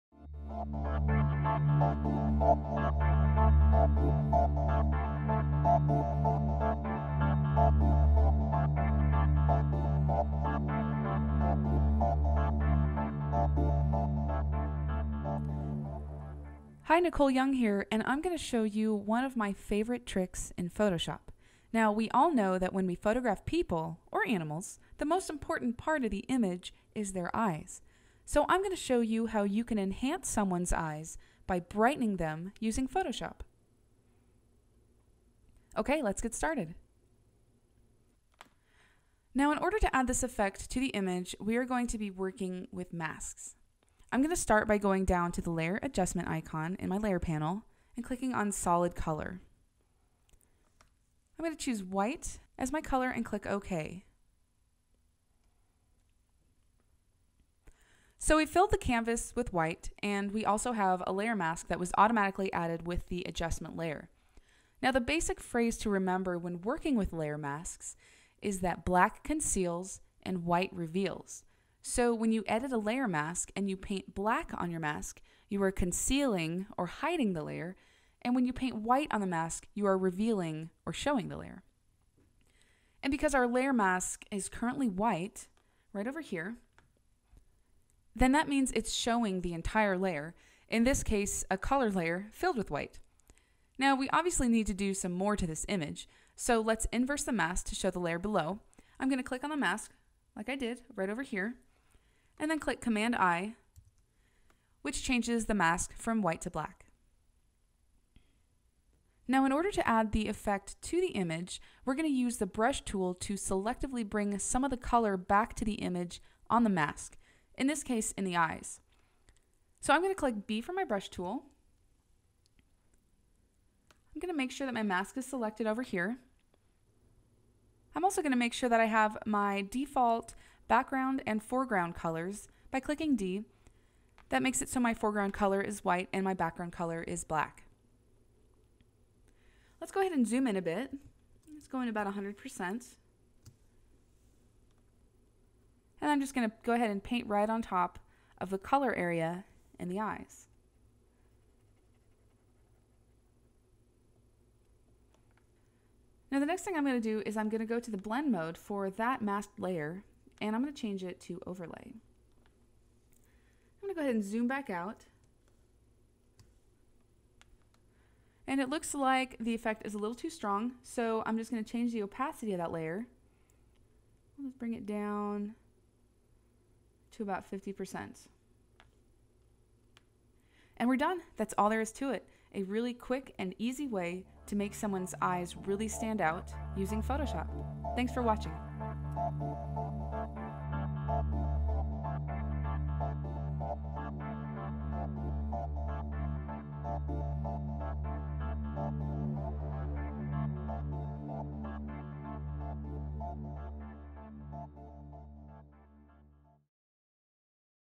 Brightening Eyes using Photoshop
Nicole Young shows how to brighten a person's eyes using layer masking in Photoshop.

(This video is an entry for the NAPP "So You Think You Can Teach Photoshop" contest